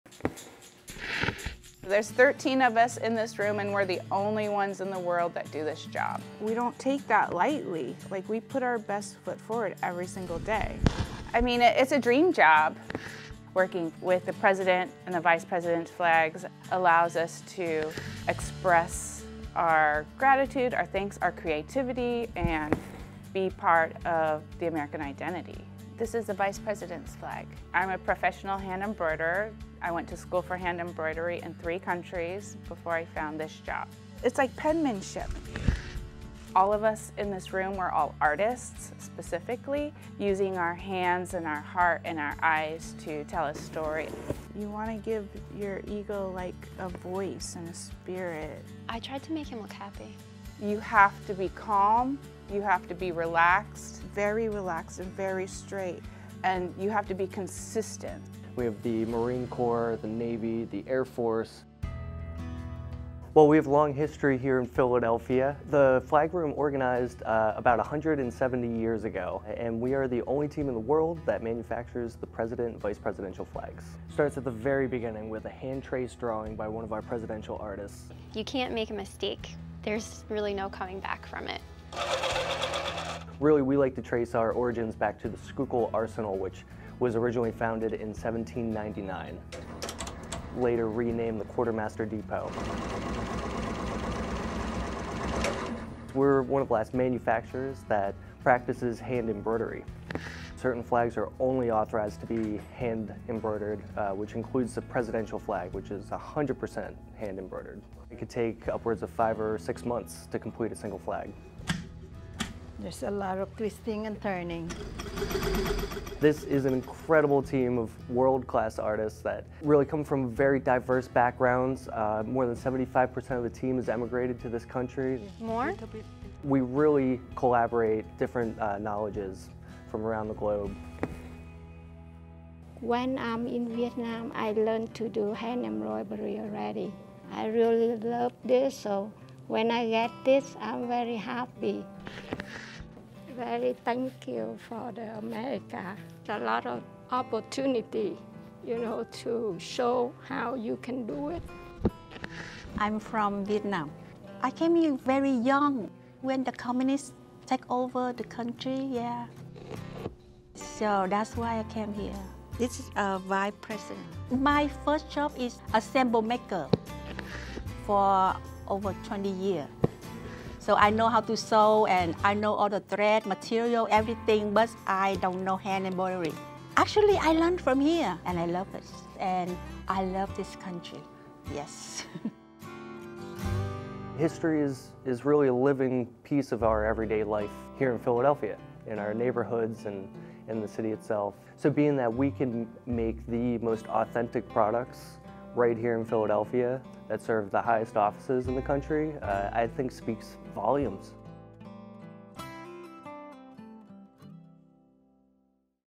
Inside the 'Flag Room': Meet the artists tasked with embroidering the President's flags
"I mean, it's a dream job," said Duwenavue Sante Johnson. "Working with the President and the Vice President's flags allows us to express our gratitude, our thanks, our creativity and be part of the American identity."

Johnson is part of a 13-person team that is exclusively responsible for manufacturing presidential and vice presidential flags.

The small workshop, known as the Defense Logistics Agency (DLA) Troop Support "Flag Room," is located in Northeast Philadelphia. The tradition had its start in South Philadelphia roughly 170 years ago, or perhaps even more.

"Really, we like to trace our origins back to the Schuylkill Arsenal, which was originally founded in 1799, later renamed Quartermaster Depot," said Adam Walstrum, the Flag Room Supervisor.

The Schuylkill Arsenal became known for outfitting the Lewis and Clark expeditions and was responsible for creating clothing and textiles for the military.

Echoes of that tradition remain loud and clear today in the Flag Room, where hand-embroiderers like Helen Hoa Nguyen spend months working on a single piece.

"When I'm in Vietnam, I learned to do hand embroidery already," she said. "I really love this so, when I got this, I'm very happy."

Nguyen is part of the majority on the dream team that immigrated to the United States.

"A lot of opportunity to show how you can do it," she said about America.

Together, the embroiderers represent one of the great contributions that Philadelphia continues to make on American history.

"History is really a living piece of our everyday life here in Philadelphia, in our neighborhoods and in the city itself," said Walstrum. "So, being that we can make the most authentic products right here in Philadelphia that serves the highest offices in the country, I think speaks volumes."

To learn more about the Defense Logistics Agency Troop Support, visit their website.